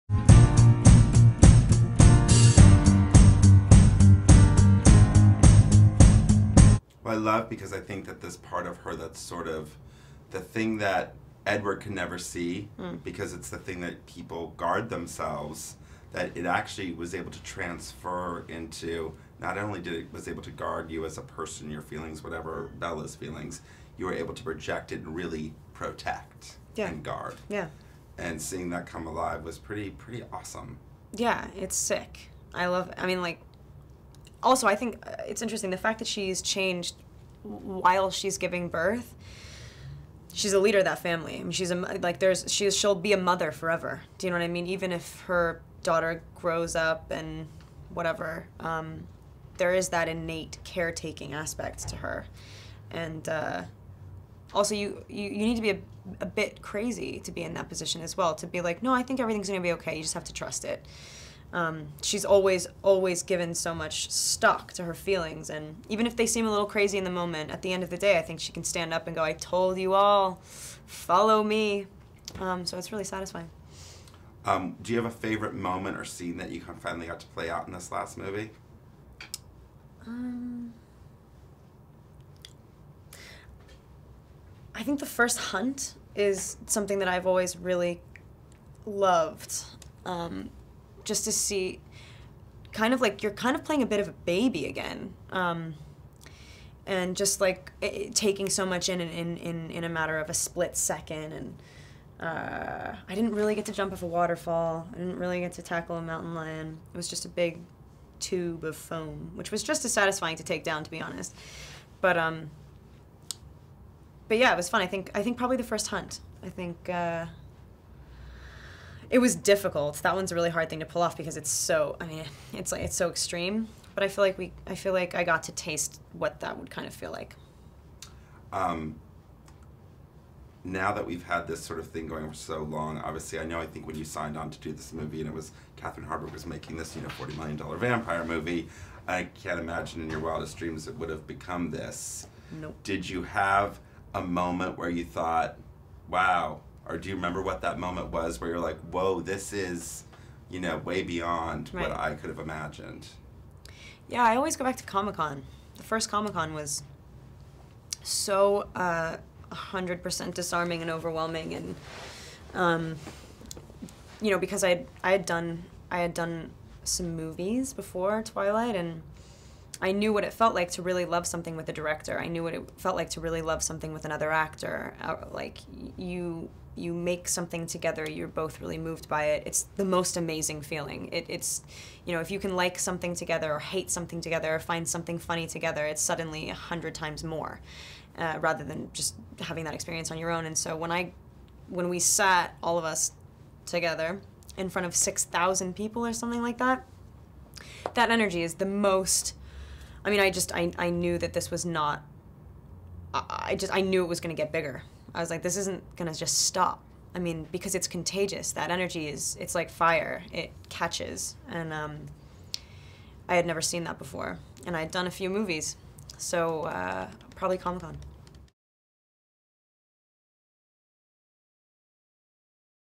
Kristen Stewart - Twilight Breaking Dawn Part 2 Interview [HD]
Kristen Stewart sits down and talks about the last installment of the Twilight franchise.